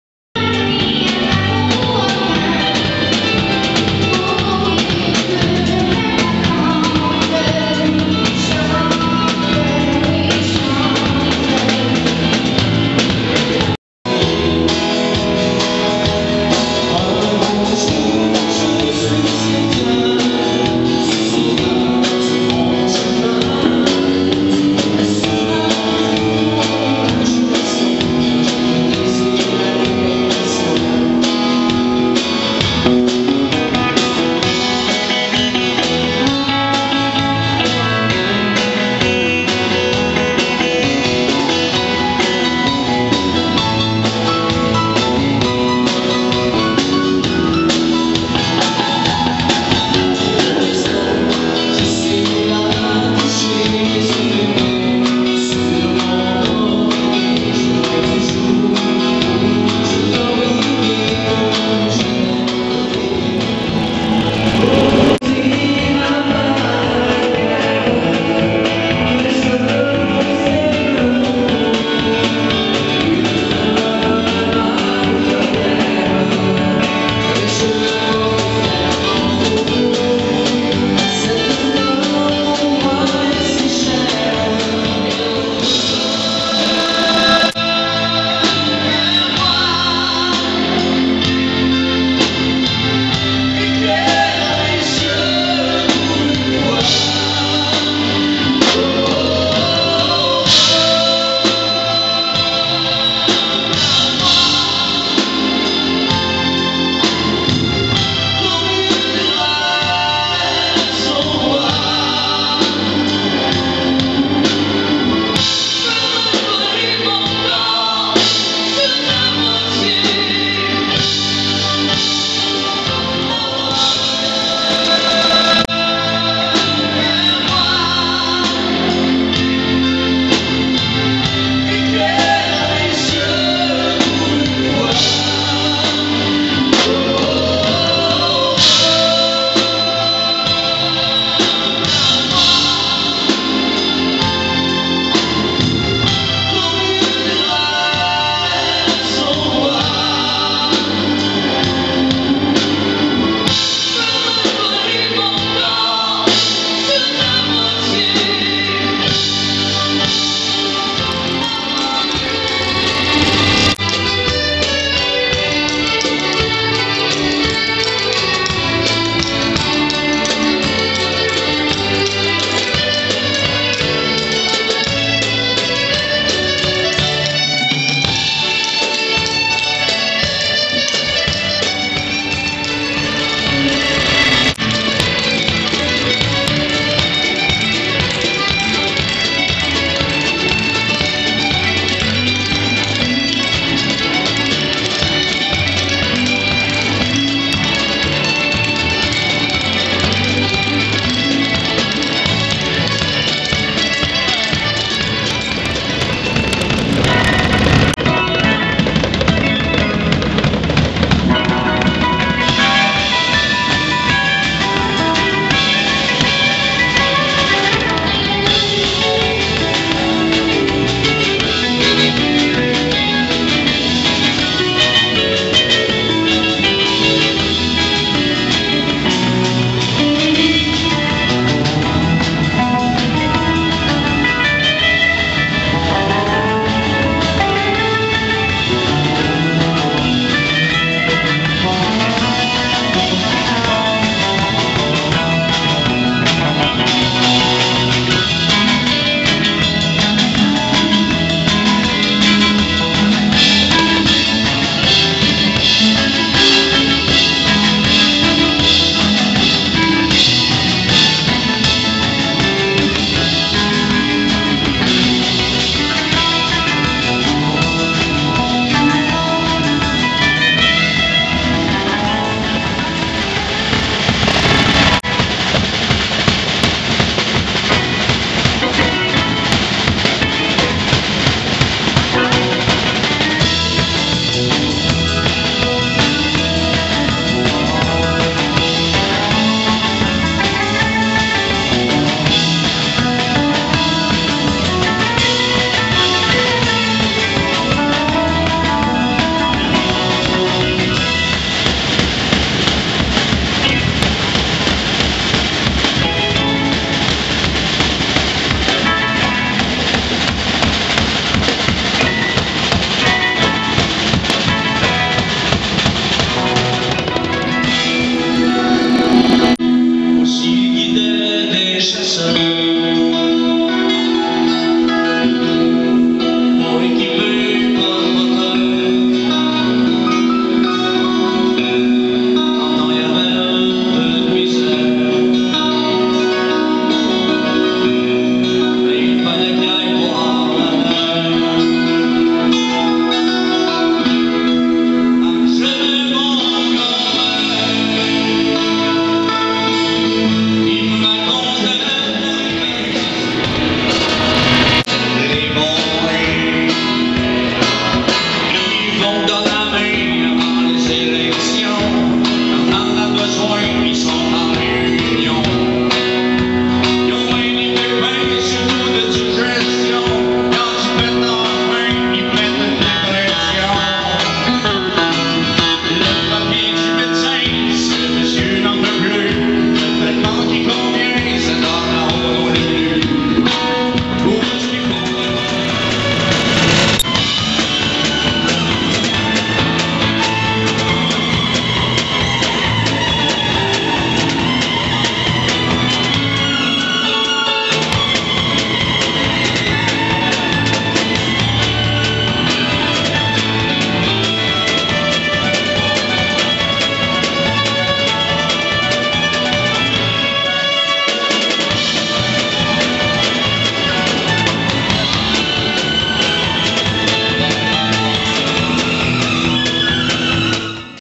Festival de Bécancour, souvenirs avec résolution 2006 mdrrrrr
Souvenir du festival Bécancour 2006 avec Jérémy Aubin, Joel Gagné, Josiane Guillemette et son frère, Nicholas, Polyphonie, Steeve Desmarais et Susy Aubin. Vidéos réalisées par Robert Ti-Bout Bouchard avec sa caméra Kodak.